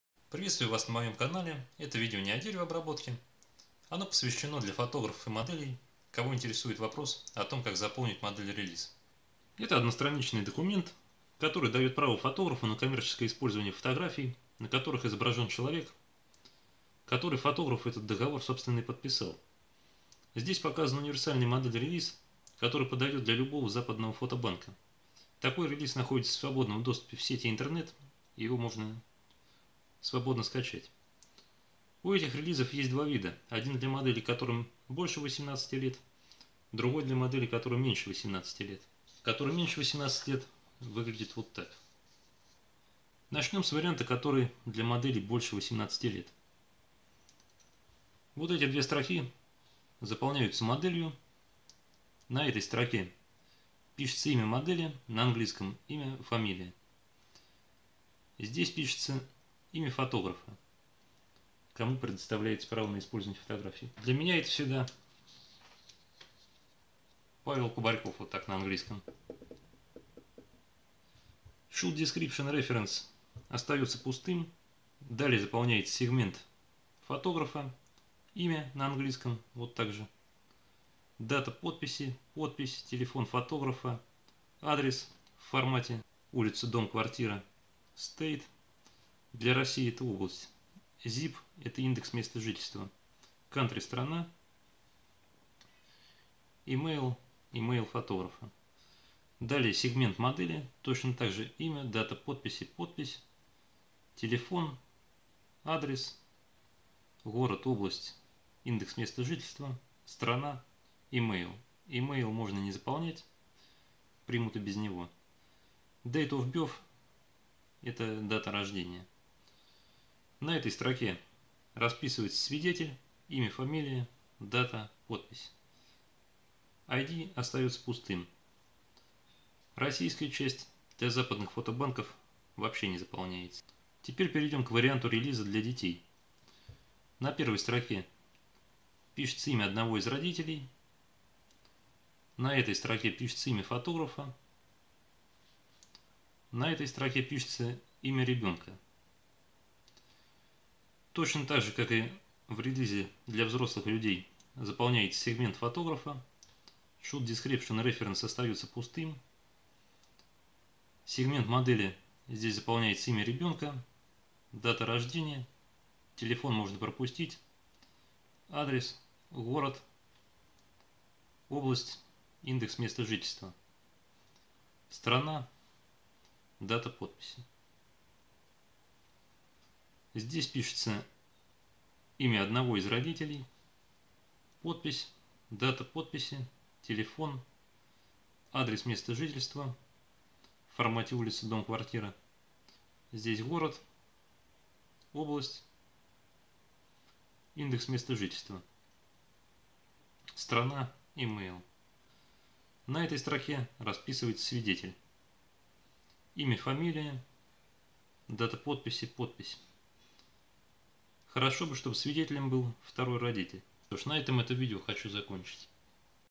Как заполнить model release взрослый и детский
На этом видео показан бланк универсального релиза модели для взрослых и несовершеннолетних и рассказано как его заполнить.